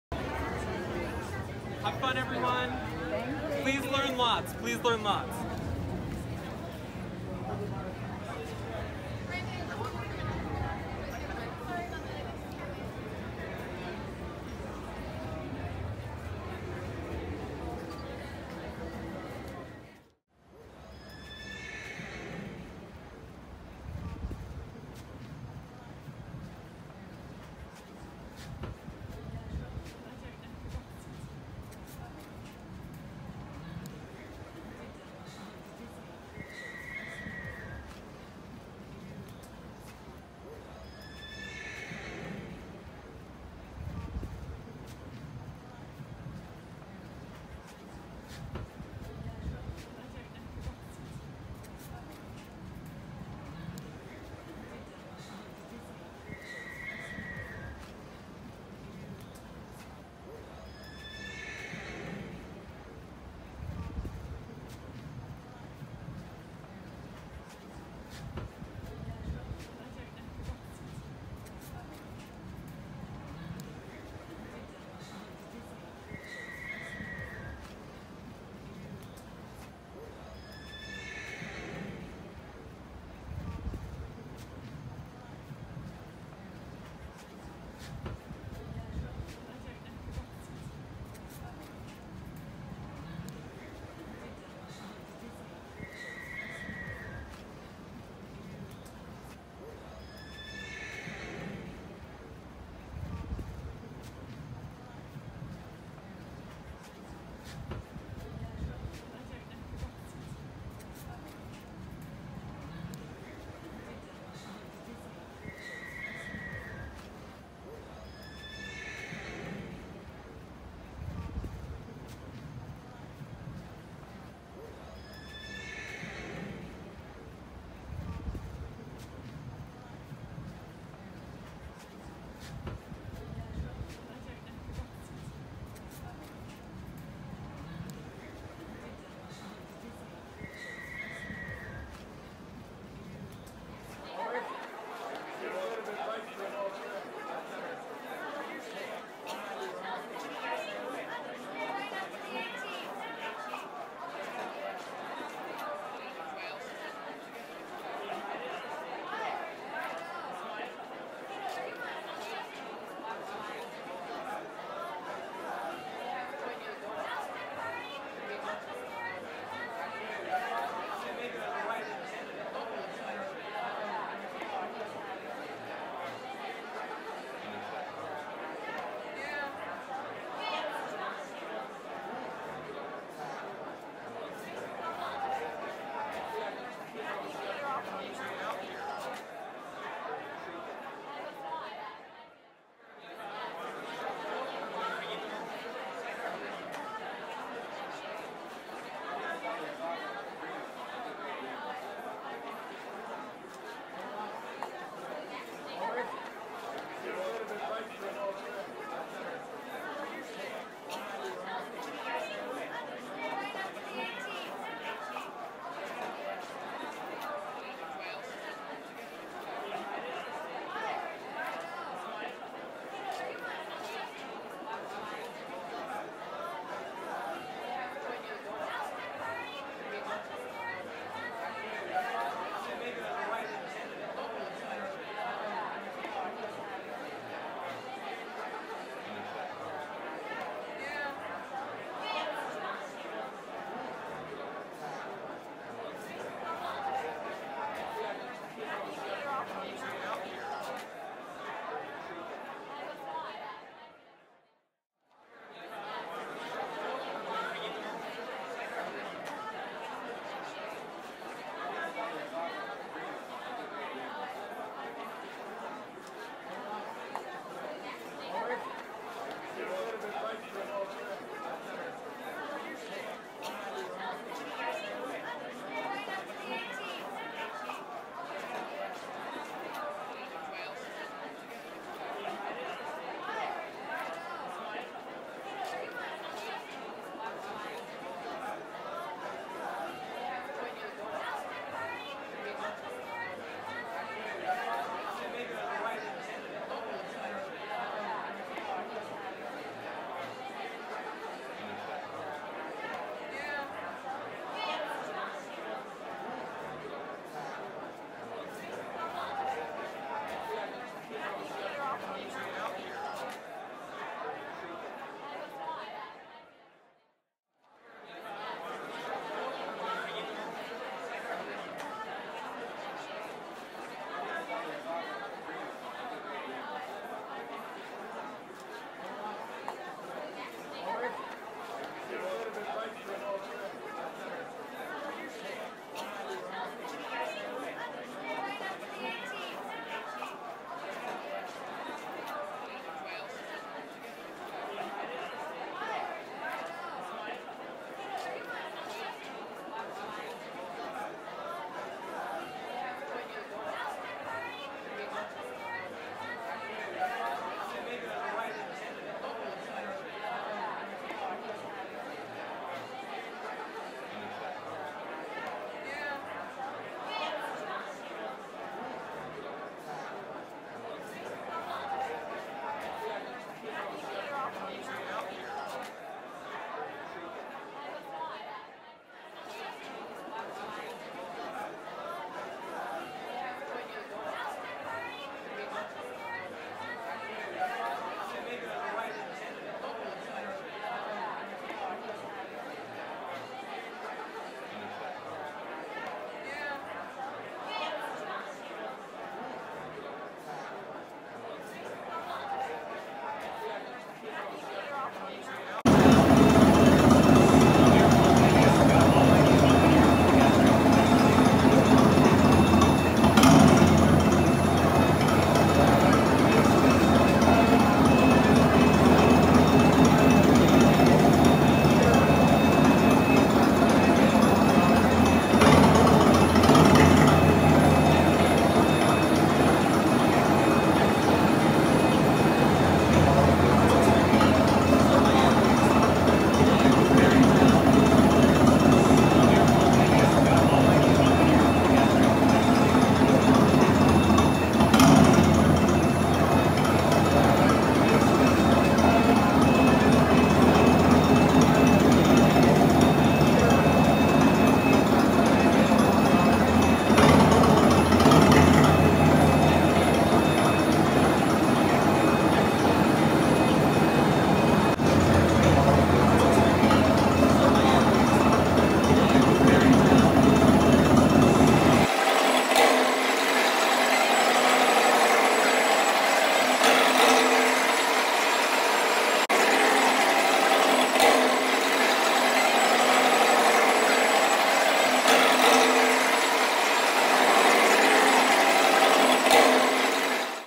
Queue of Hagrid's Magical Creature Motorbike Adventure-Fila montaña rusa Hagrid-Harry Potter
Hagrid's Magical Creatures Motorbike Adventure is the new roller coaster in The Wizarding World of Harry Potter-Hogsmeade at Universal Orlando Resort.//La Hagrid's Magical Creatures Motorbike Adventure es la nueva montaña rusa de Hagrid en el Mundo de Harry Potter, en Islands of Adventure de Universal Orlando Resort.